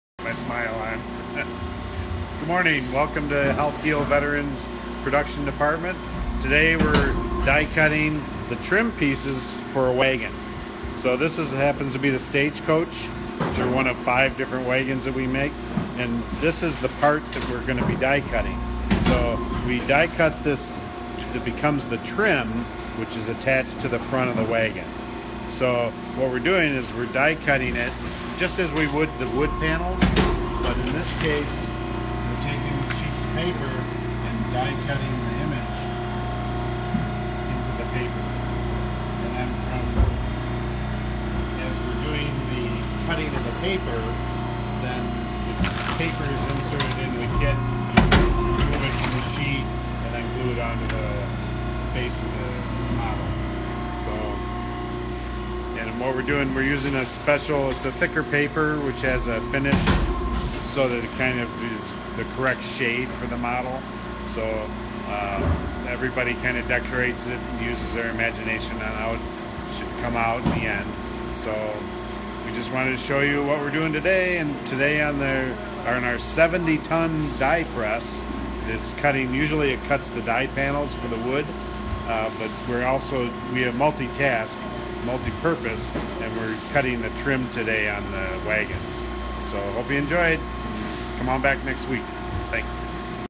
Chip's Craft Corner - Using the Die Cutter for the trim on the Wooden Wagon Kit
Today we are using the die cutting machine to cut the trim for the Wooden Wagon Kit.